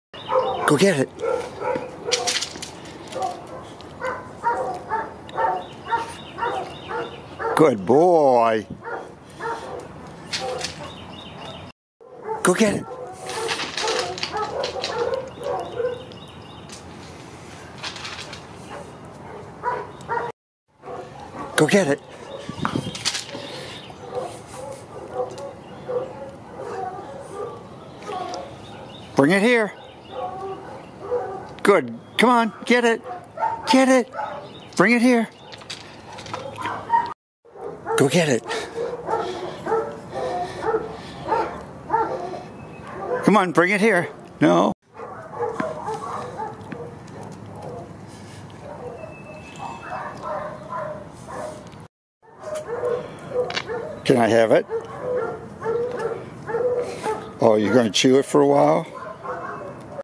Moose #2
Moose #2 is a 3 year old black neutered male